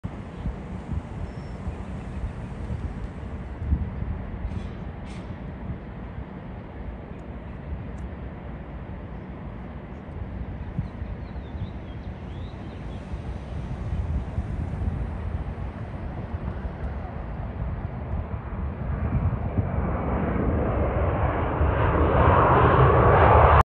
Southwest Airlines 737 MAX 8 Landing At Raleigh/Durham International Airport.
Huge shoutout to:

@thejazzguy13 @bp22470 @TripleSGames @Travel Mango Aviation @CenTexAviation @jetblue @npccommander72
@KristinaSchiano @AveryDrummer @Abdul Abusada Aviation @omarpercolla3089 @OmerMazaltarim